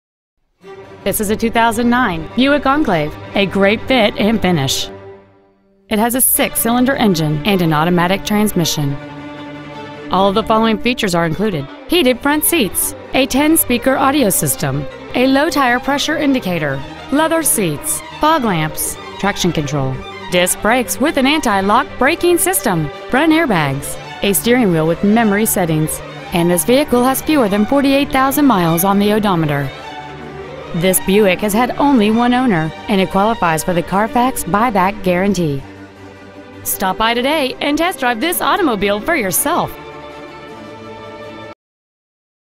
Used 2009 Buick Enclave Miami FL
Year : 2009
 Make : Buick
 Model : Enclave
 Engine : dohc vvt 24v v6
 Trans . : automatic
 Exterior : brown
 Miles : 47,500
 Interior : brown

 Lehman Mazda